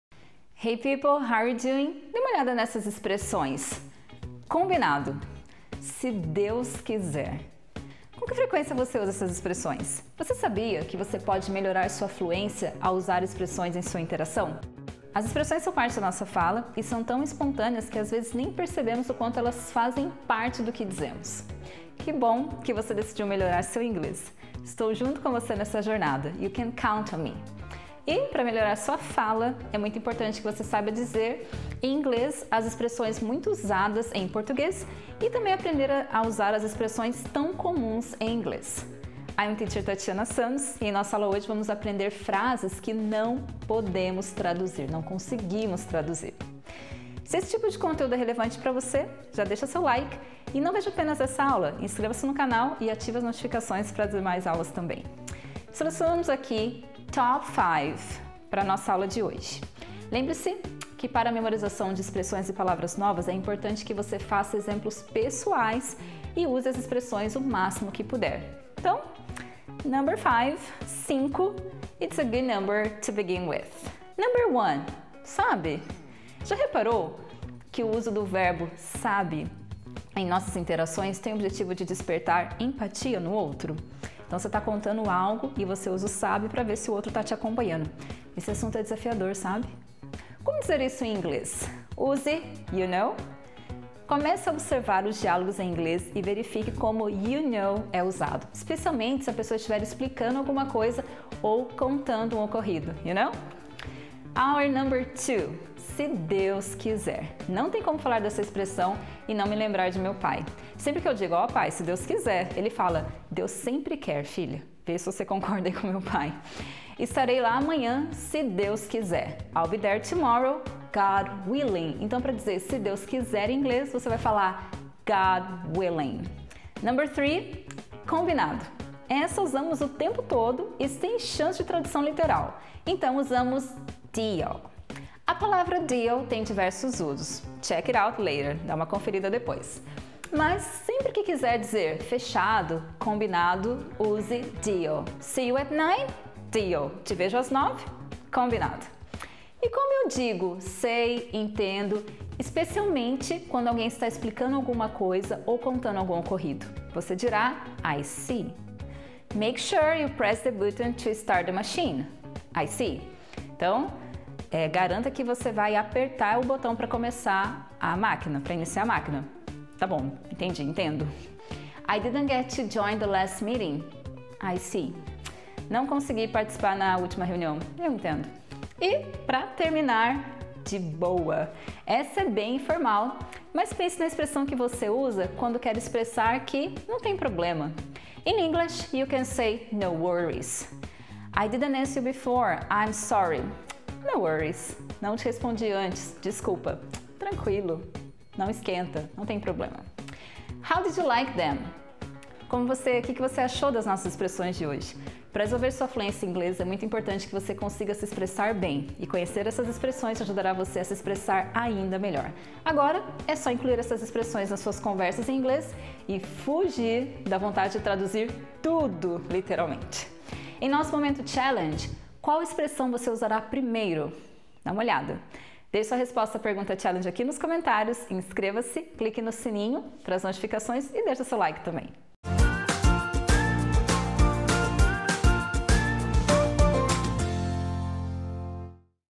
Como dizer “Se Deus quiser!” em Inglês?
Você tem vontade de soar mais natural falando em inglês?
Hoje vou te ensinar algumas expressões que sempre usamos no português mas que não tem uma tem tradução exata, e que vão te ajudar a melhorar na sua fluência

Qual expressão você vai usar primeiro?